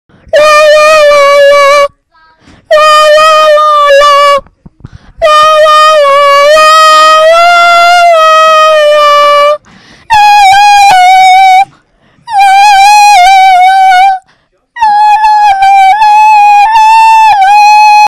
aniboooms1 is skating again
its me skateing after 4 months of not skating and i big spin kickflip and  verial kickflip and sing a retarded song la la la la la la la la la la la laaaaa la la